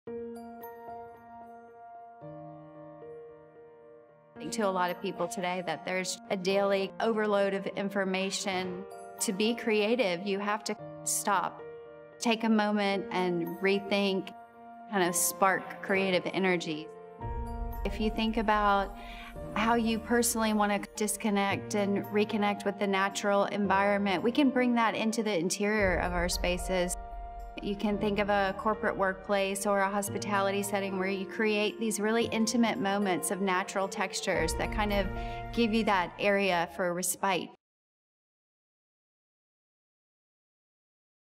Off the Grid || Mindfulness in the Workplace
Mindfulness and creativity go hand-in-hand. Off the Grid brings a moment of natural beauty into the workplace for inspired productivity.